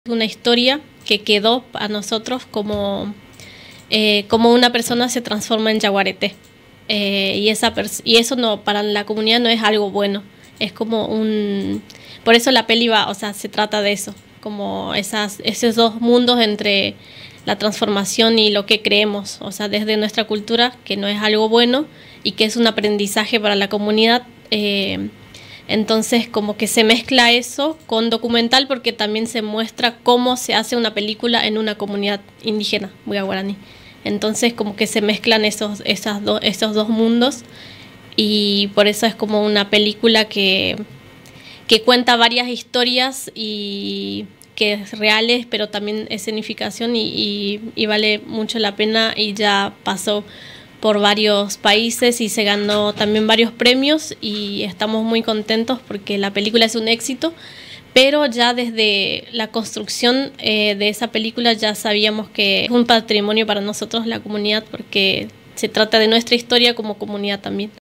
La leyenda del joven que se transformó en yaguareté
🎥 En el marco del festival Oberá en Cortos, se presentarán películas que celebran la diversidad cultural. Una de ellas es “La Transformación de Canuto”, una obra que lleva 7 años en proceso y que mezcla documental y ficción para contar una historia real acaecida en la comunidad Tamanduá.